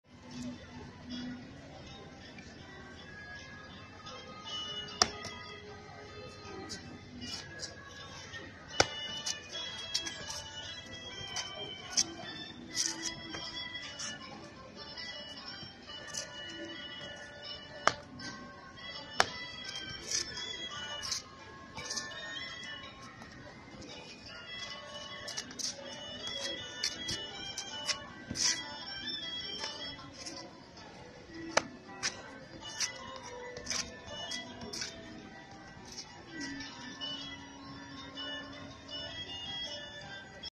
螳螂拳 楊家超老師 攔截 2017年
山東省聊城市に傳わる螳螂拳です。
系譜は不明です。

原題：「螳螂拳拦截（聊城杨家超）」